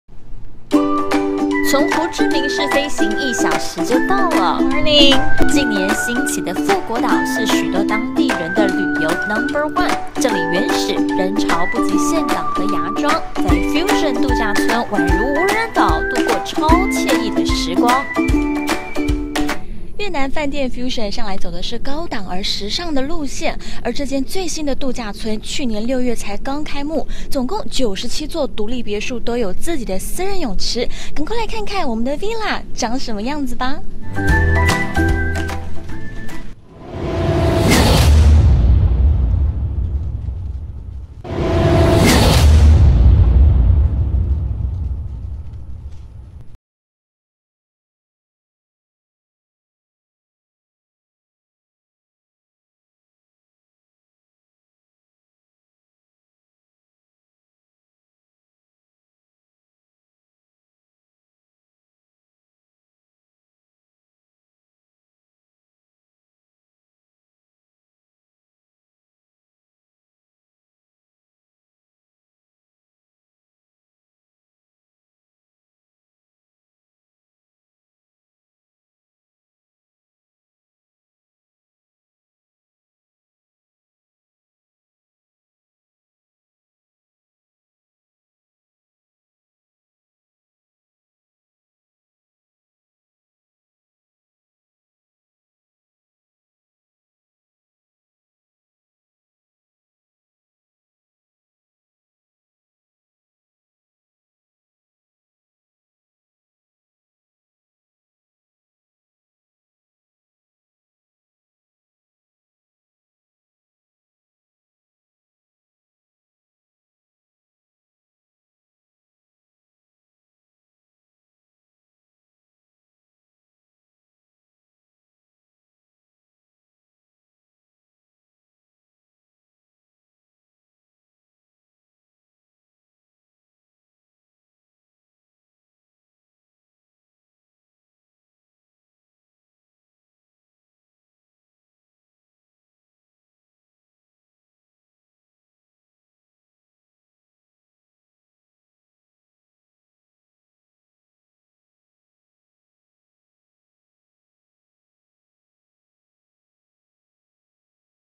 PHU QUOC FUSION RESORT/HEAVEN LIKE MUST WATCH#phuquoc#fusionresort#vietnam#resort#beach#hochiminh
Fusion Resort渡假村在2017年開幕
坐落在西北岸
人潮不如東南岸熱鬧
但是如果你嚮往的是非常靜謐的度假空間
這裡可是首選